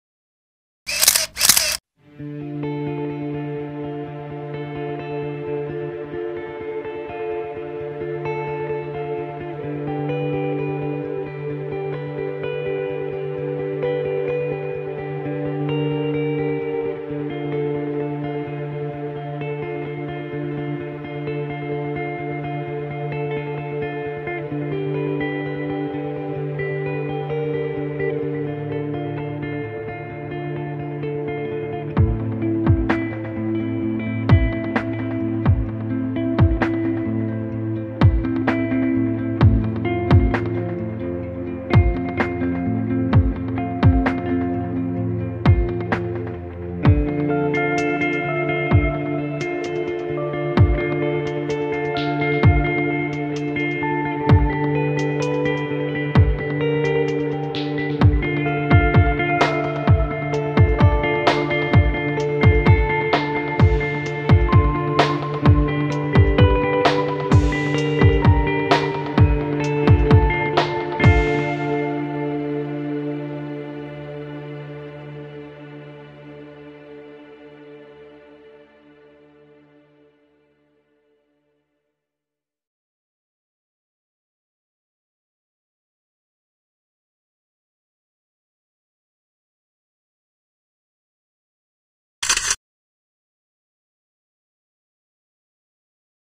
Photographies de Nathalie
Plus de 300.000 photos accumulées au cours de 15 ans de voyage.
En voici un tout petit échantillon pour le plus grand plaisir des yeux.